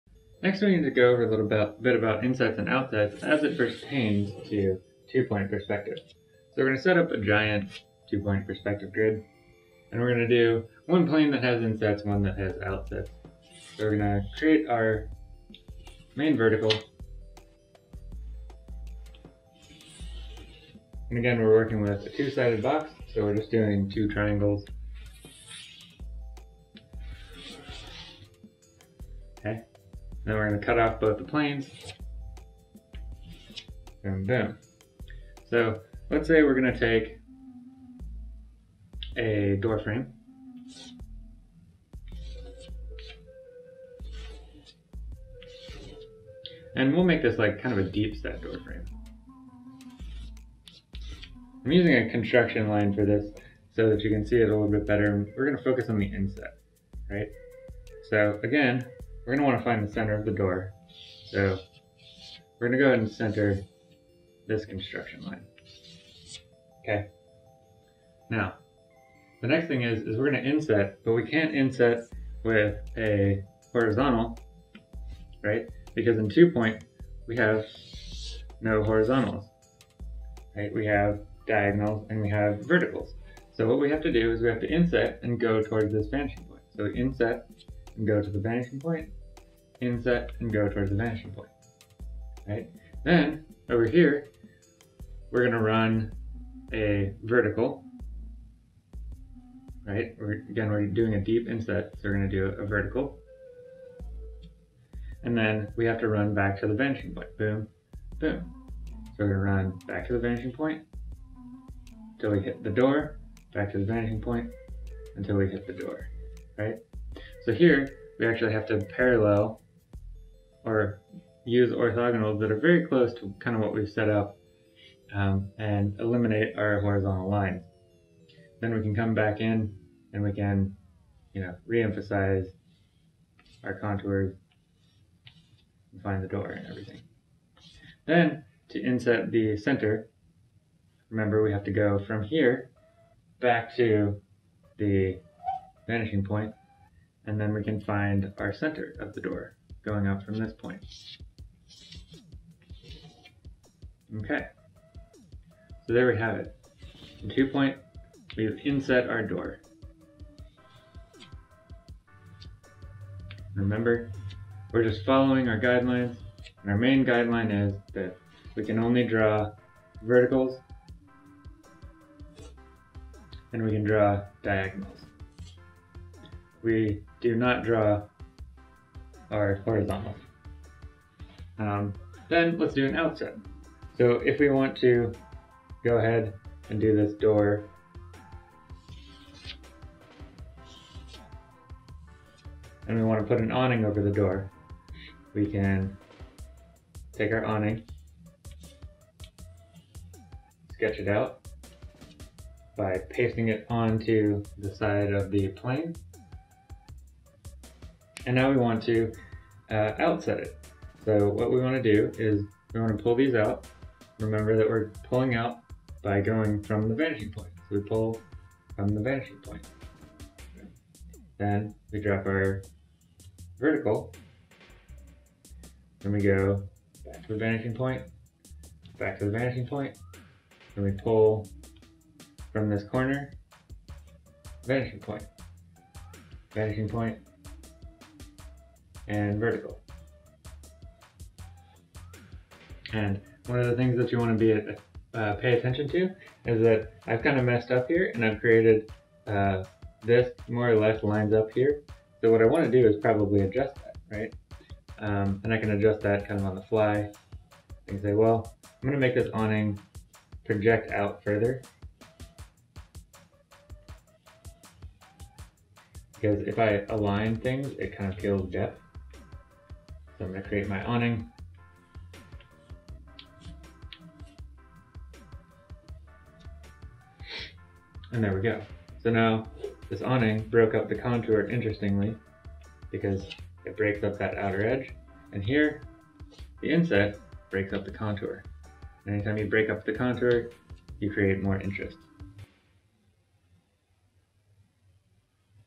Intro to Perspective: Inset and Outset in 2-Point
This video goes over insetting and outsetting doors and awnings in 2-point perspective. The concept will apply to all the various architectural details you may encounter.

Materials I like:
Prismacolor Premier Colored Pencils
Strathmore lightweight sketch pad